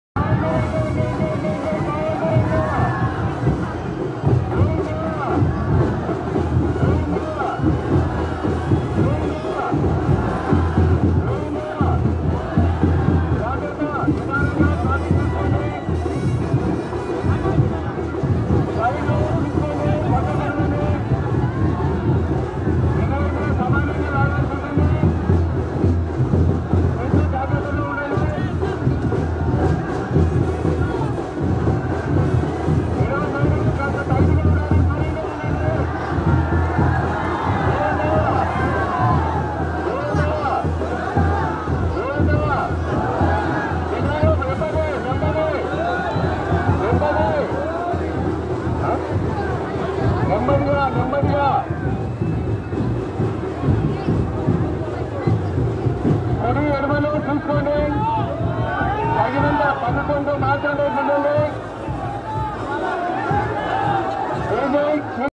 కోవెలకుంట్ల శ్రీ పాండురంగ స్వామి రథోత్సవం 2022
శ్రీ పాండురంగ స్వామి బ్రహ్మోత్సవాలు, కోవెలకుంట్ల టౌన్, నంద్యాల జిల్లా. 2022